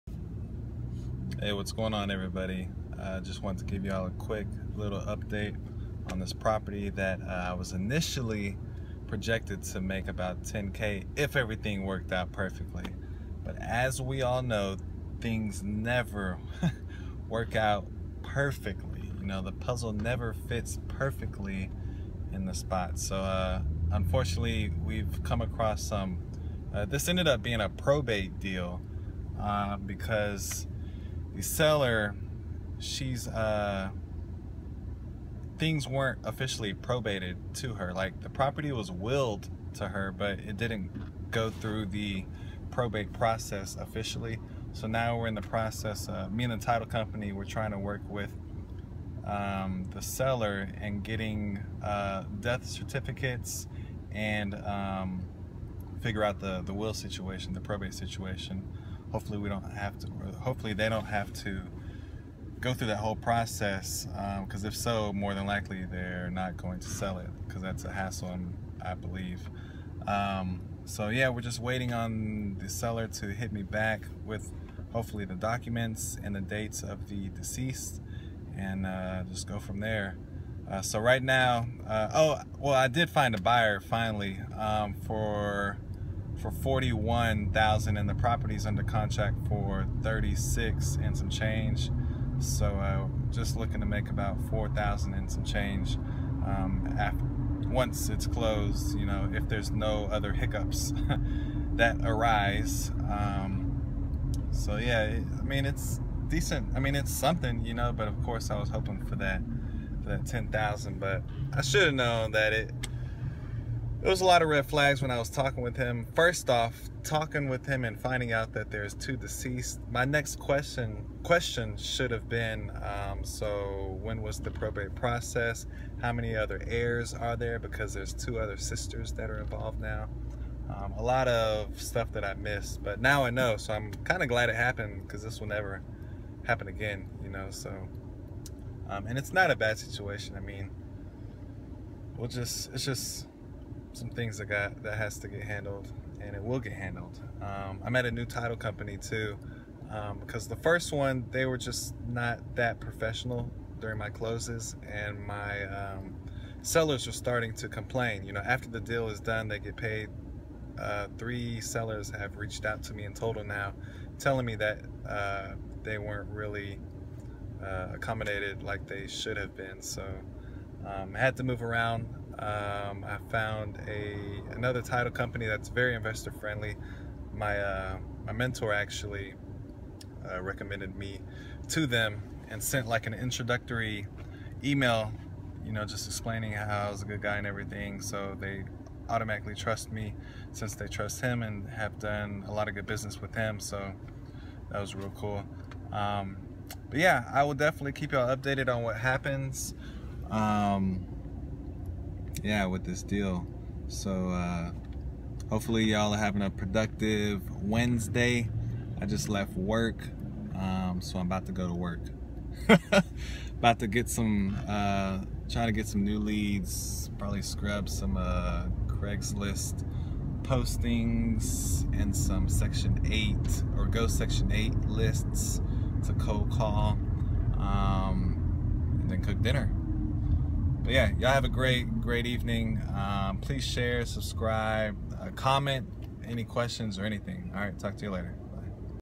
Things don't always go according to plan - Real Estate Wholesaling
Update on a current deal. Things don't always go according to plan.